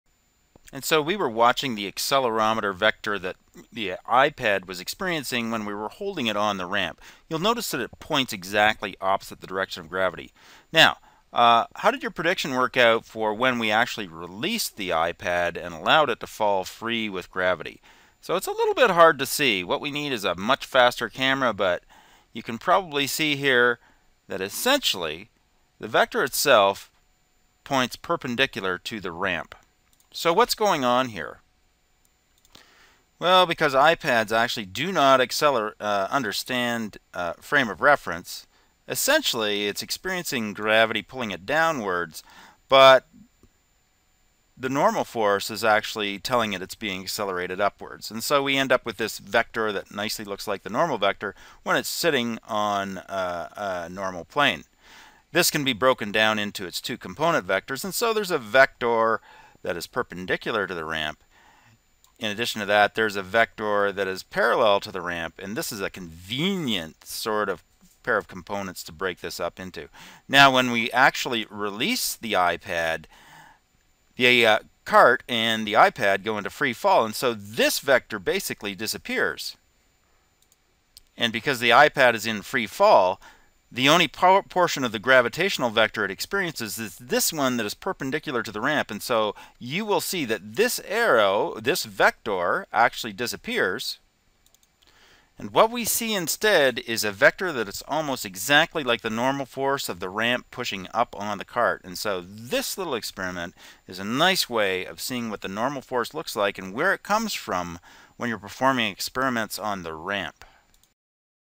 Accelerometer On An Inclined Plane
What happens when we release an Accelerometer on a cart on an inclined plan and allow it to "free fall" in one direction? We essentially see the "normal" vector.